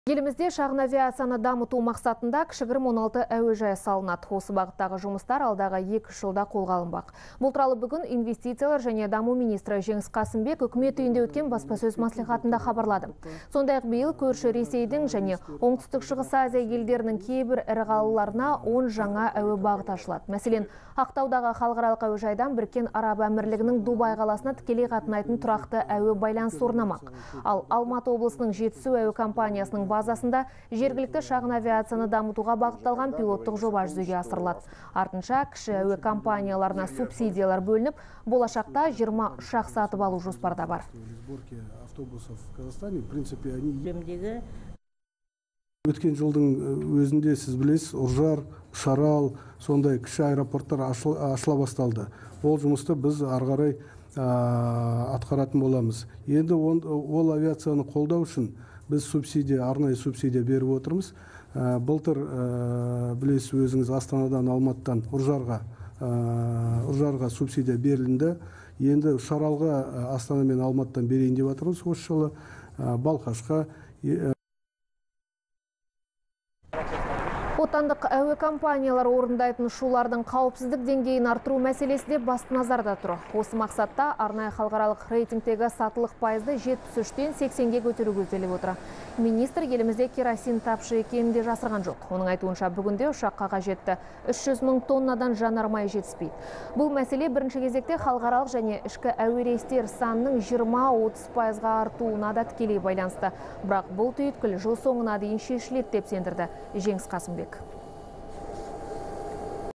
Елімізде шағын әуежайлар саны артады
Елімізде шағын авиацияны дамыту мақсатында кішігірім 16 әуежай салынады.
Осы бағыттағы жұмыстар алдағы 2-3 жылда қолға алынбақ. Бұл туралы бүгін Инвестициялар және даму министрі Жеңіс Қасымбек Үкімет үйінде өткен баспасөз мәслихатында хабарлады.